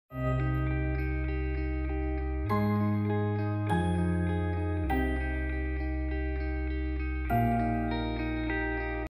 Sweet 9 Roses + 2 Love Balloon Bouquet and LED in the Transparent Gift Box
9 Soap roses with unique colors + 2 love Balloon bouquet in the transparent box with LED